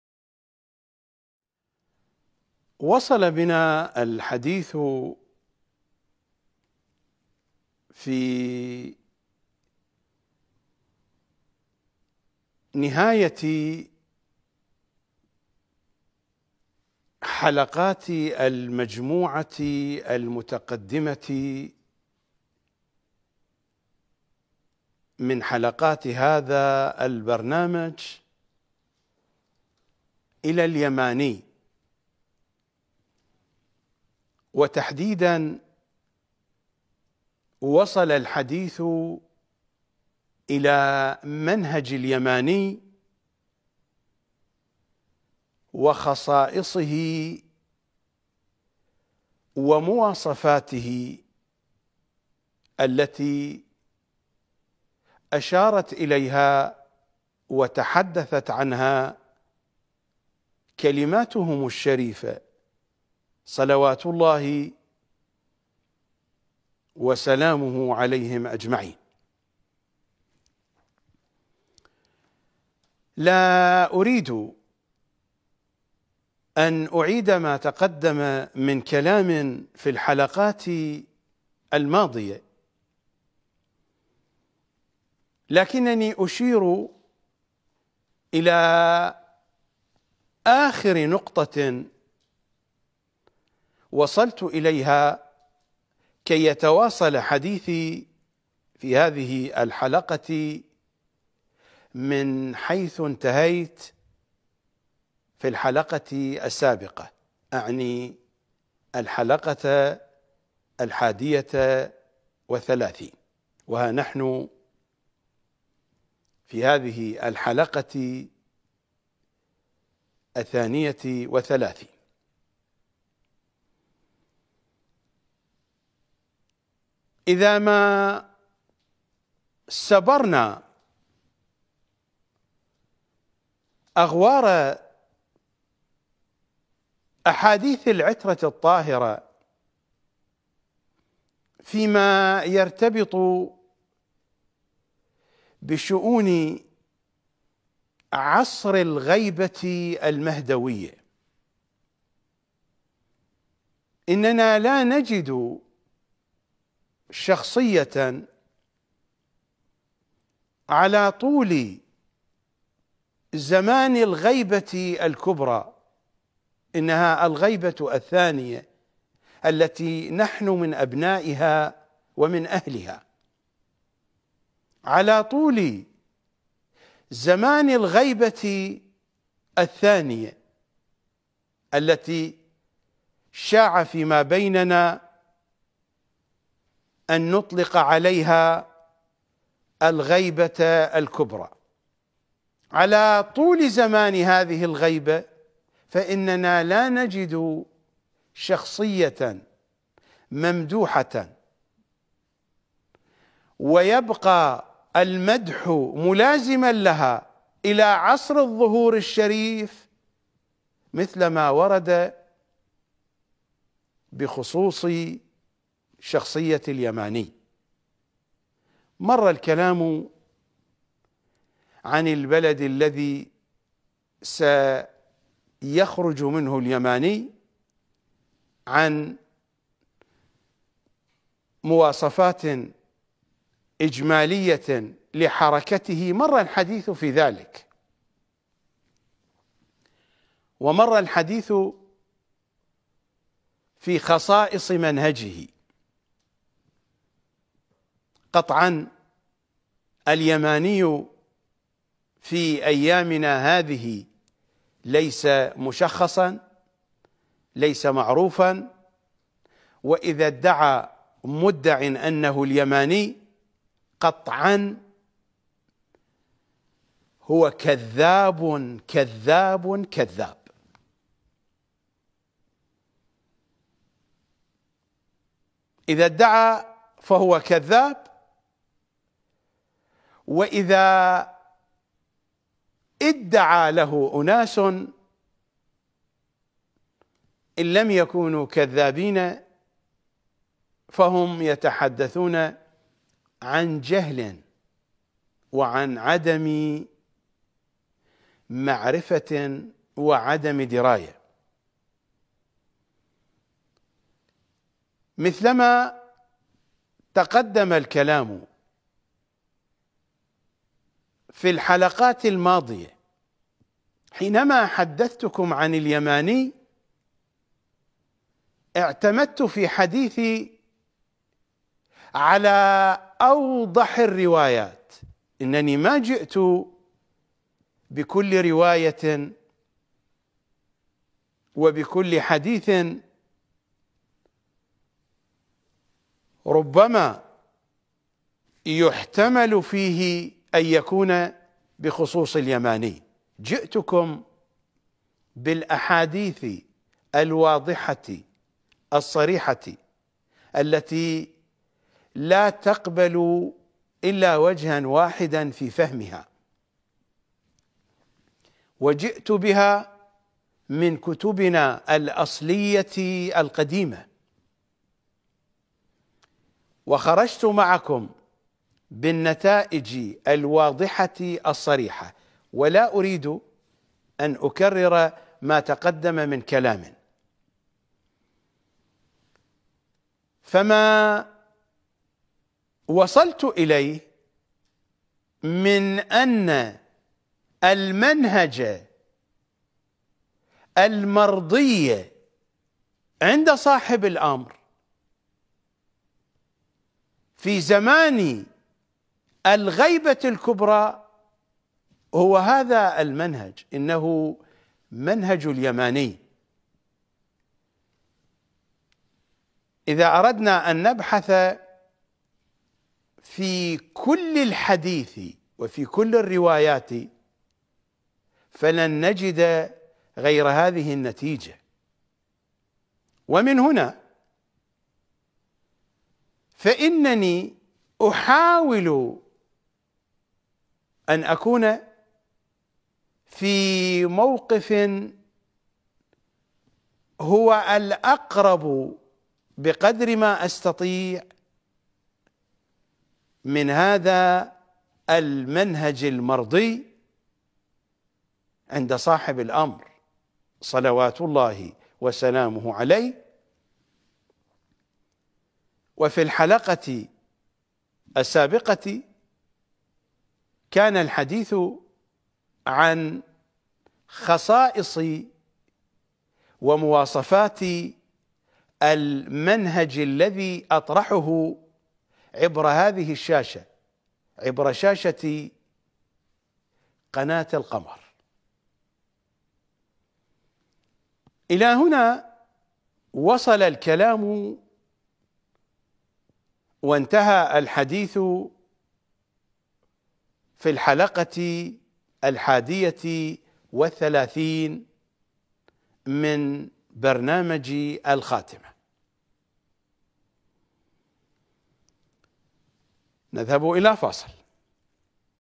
المنهج المرضيَّ عند صاحب الأمر في زمان الغيبة الكبرى هو منهج اليماني - الشيخ الغزي
ولا تنسى بالشتراك في القناة 🙏🌹